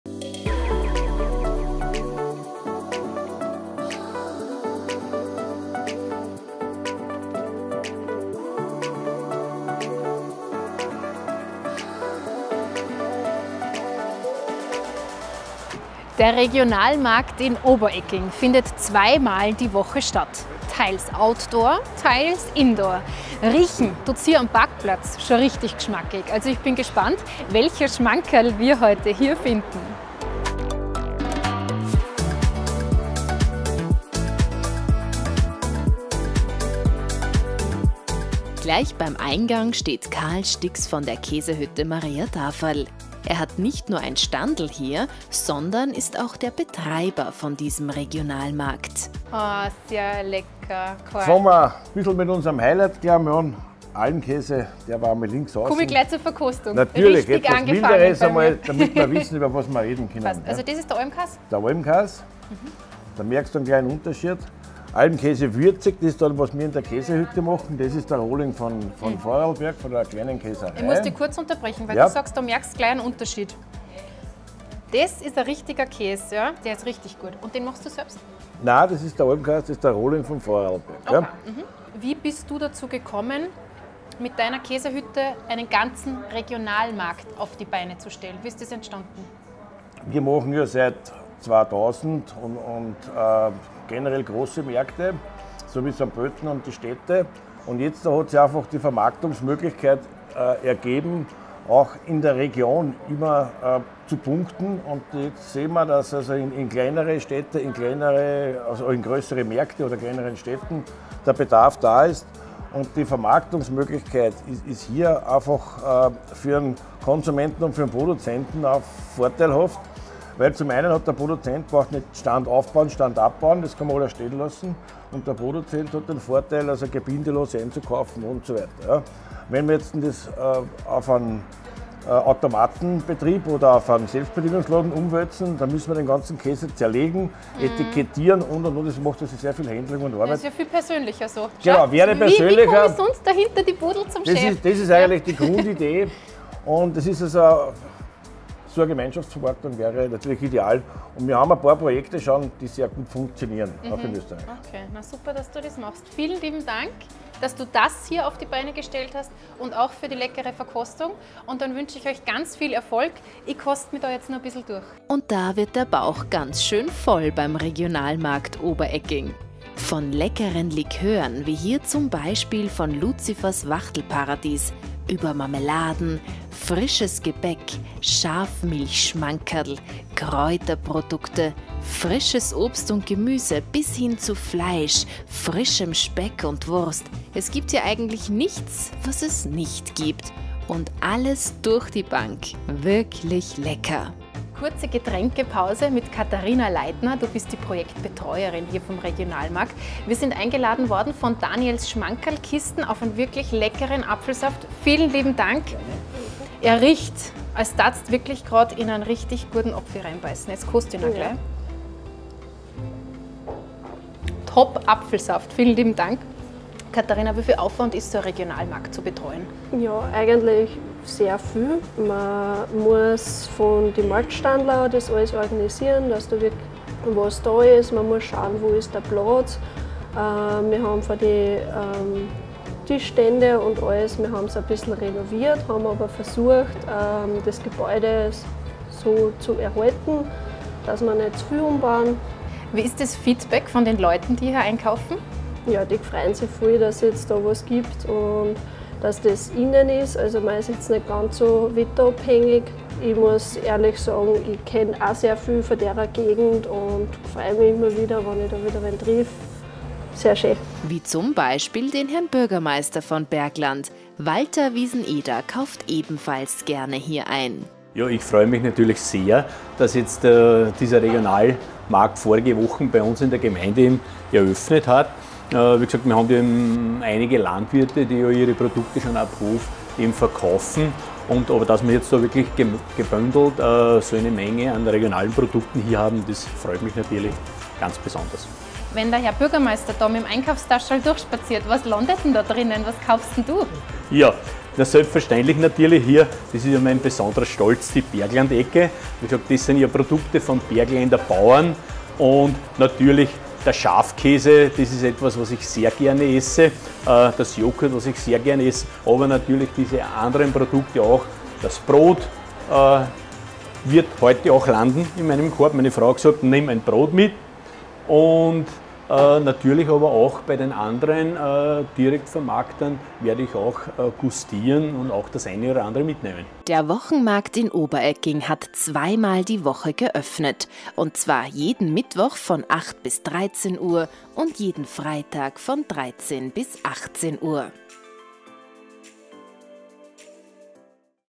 Einkaufen am Wochenmarkt: In Oberegging gibt's regionale Spezialitäten
In Oberegging gibt es ab jetzt einen ganz besonderen Wochenmarkt. Im Gebäude des Gasthofs zur Alm wurde der alte Speisesaal zu einem Marktplatz umgebaut. Somit kann man indoor zweimal in der Woche ganz regional einkaufen - Mittwochs von 08:00 bis 13:00 Uhr und Freitags von 13:00 bis 18:00 Uhr. GemeindeTV hat sich am Wochenmarkt umgesehen.